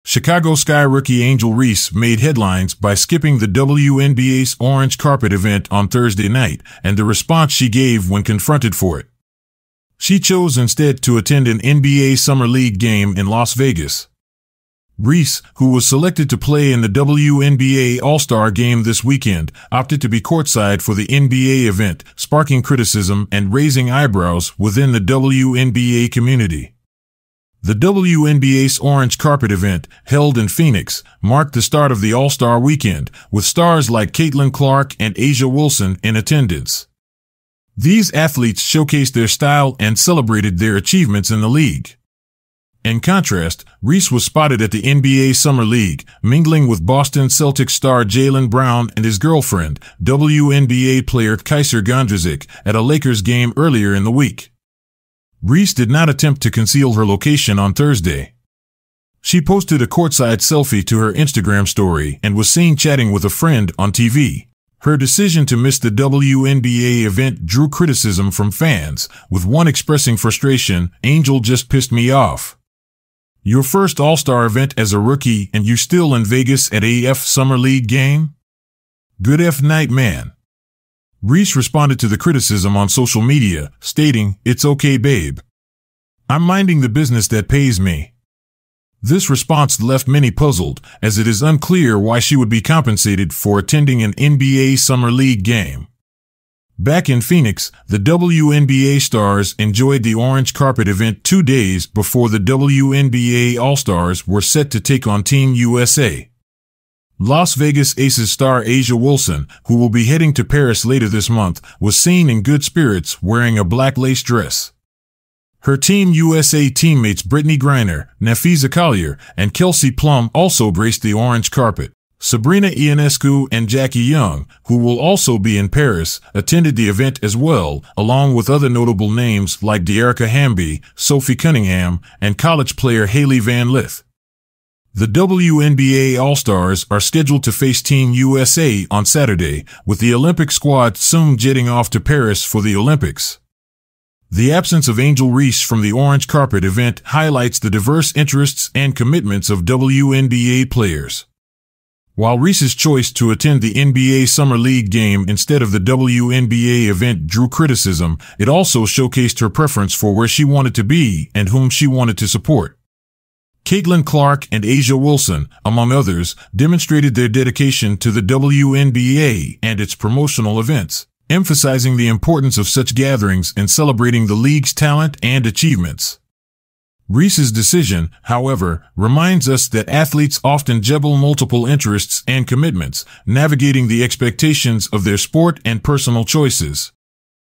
Angel Reese's RUDE Response when asked why she missed WNBA All Star Orange Carpet
Angel Reese missed the WNBA All Star Orange Carpet Event and when asked why she did so by a fan, she had a very interesting answer. All the other All Stars and USA Women basketball team including Caitlin Clark, A'ja Wilson, Diana Taurasi among others  were present for the occasion.